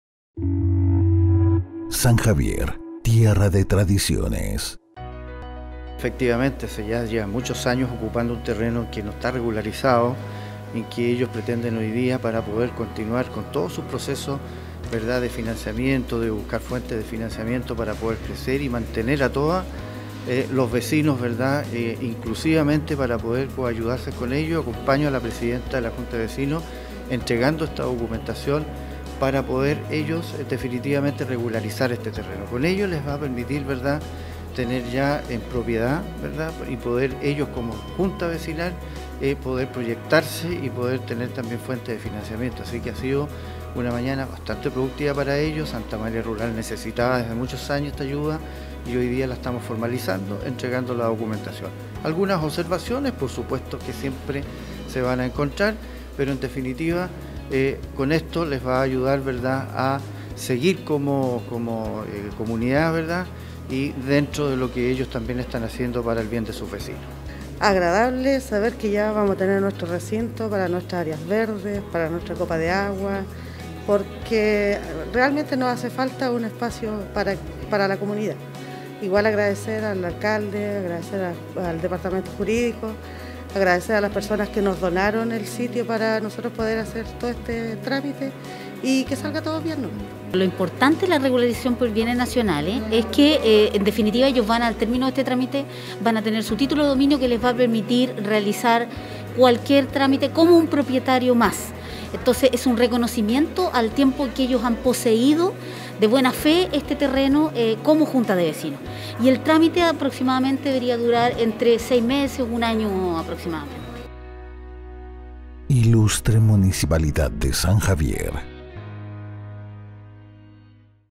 JJVV Santa Amalia Rural, inicia regularización de su terreno
Hoy culminó la postergación que por años han vivido los integrantes de la JJVV de Santa Amalia Rural en relación al título de dominio del terreno donde se emplaza su sede social y la copa de agua del sector. Hasta el edificio de Bienes Nacionales en Talca llegaron esta mañana la Presidenta de la Junta de Vecinos Patricia Valdés en compañía del Alcalde de la comuna de San Javier Jorge Ignacio Silva, el equipo jurídico municipal y la encargada territorial para entregar la carpeta de documentos con la cual se inició tan esperado proceso.

“Para nosotros es realmente agradable, saber que vamos a tener nuestro recinto para nuestras áreas verdes , para nuestra copa de agua. Agradecer el apoyo del alcalde, del municipio, a gradecer a las personas que nos donaron el terreno” Patricia Valdés, pdta. JJVV Santa Amalia Rural

“llevan muchos años ocupando un terreno que no está regularizado y que ellos pretenden para continuar con sus procesos y buscar financiamiento para proyectos. Esto les va a permitir proyectarse como comunidad, Santa Amalia Rural por años necesitaba esta ayuda y hoy lo estamos formalizando por el bien de los vecinos” Alcalde Jorge Ignacio Silva.

Entre 6 y 1 año demorará el trámite y uno de los primeros proyectos que ya se proyectan los vecinos será postular a subsidios y proyectos que les permitan alcanzar el anhelado proyecto de agua potable para toda su comunidad.